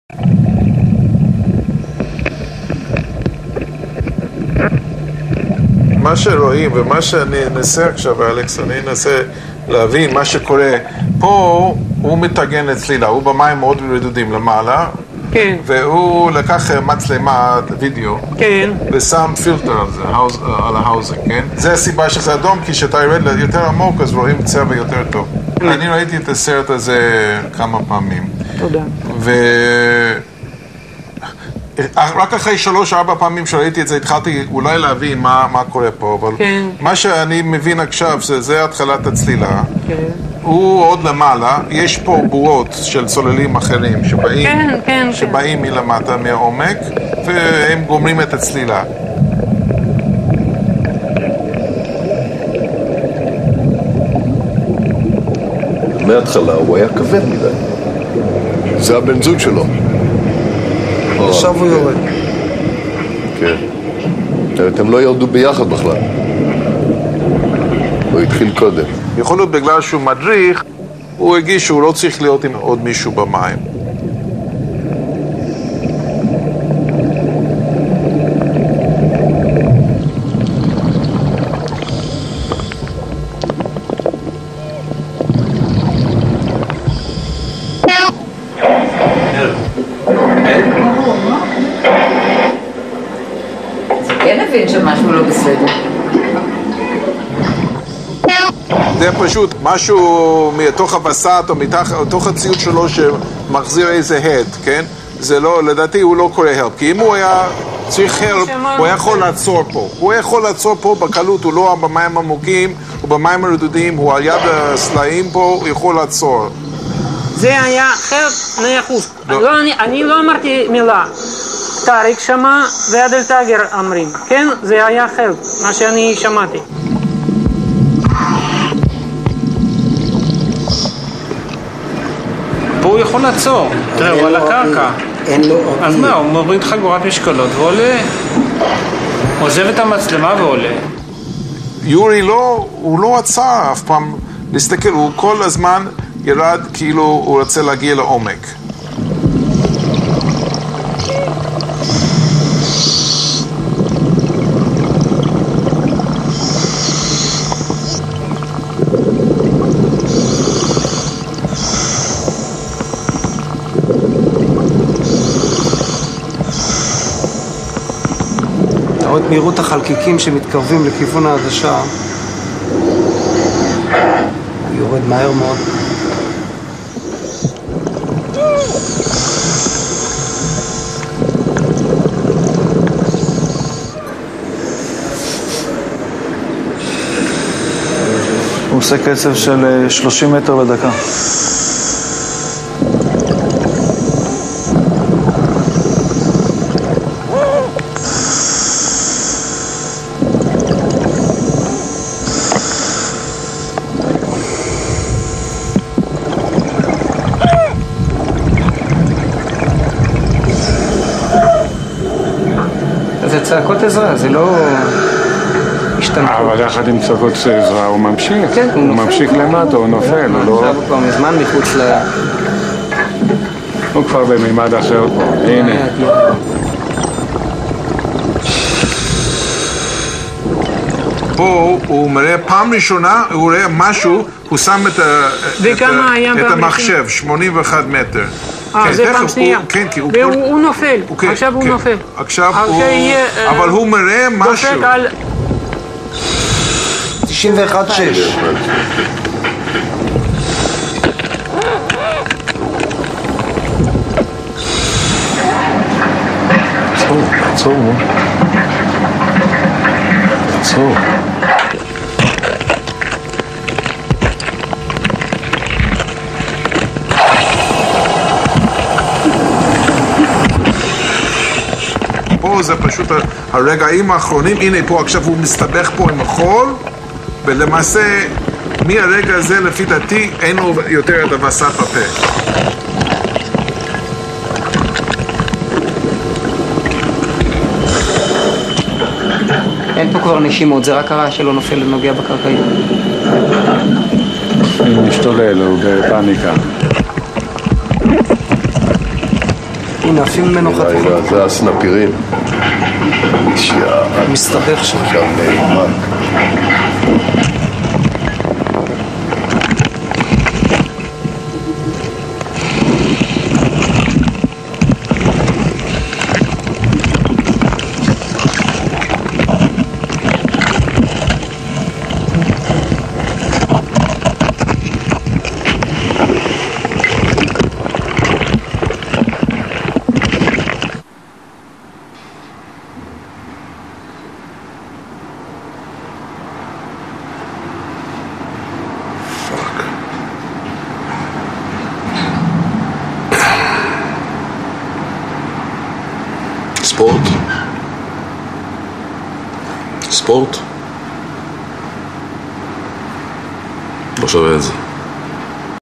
Accidente Intoxicación OXIGENO
Fatal Diving Accident caught on Tape